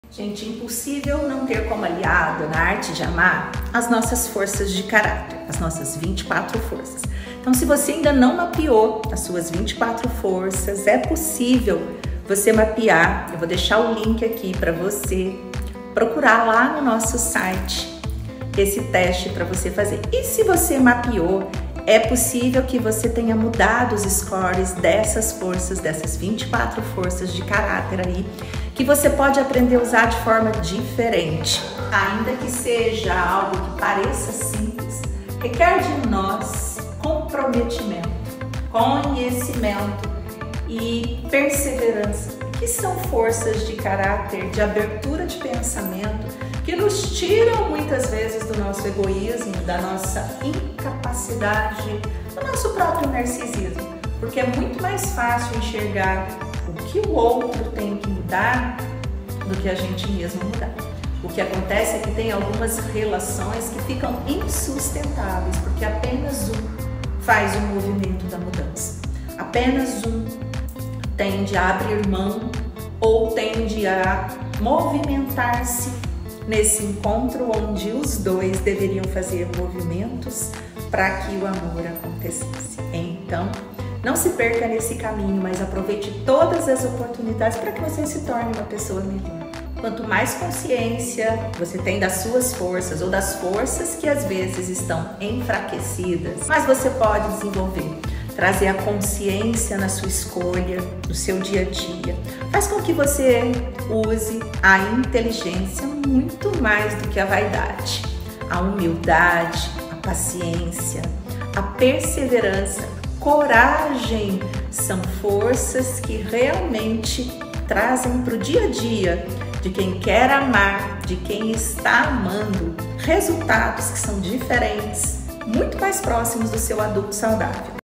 É por isso que pessoas que amam são mais saudáveis.
A arte de amar importa e diminui sua ansiedade.

Agende sua consulta agora.